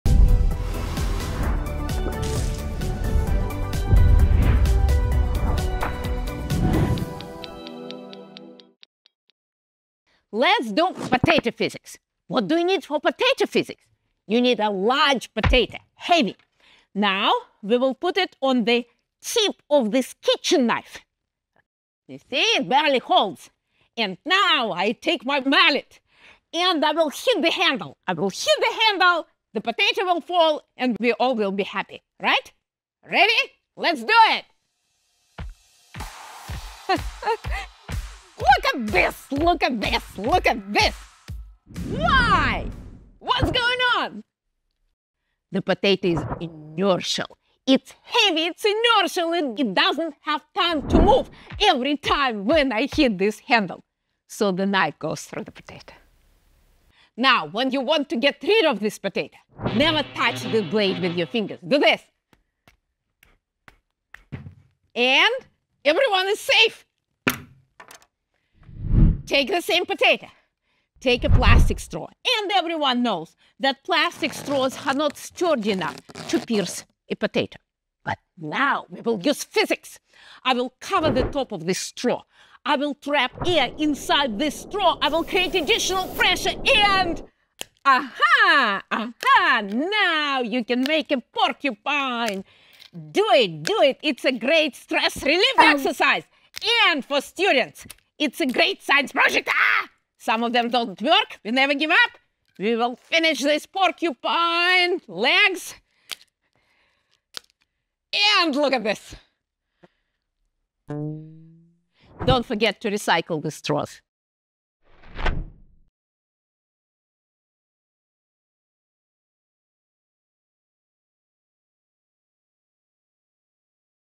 Dr. Tatiana Does Potato Physics! | Physics in Minutes
French fries, mashed potatoes, baked potatoes, porcupine? Using physics, plastic straws and pressure, Dr. Tatiana from @tamuphysastr can make a potato into anything. She can send a potato shooting up a knife with the power of inertia or push straws into the potato skin effortlessly by strategically using pressure.

This program and channel are made possible by viewers like you. Thank you!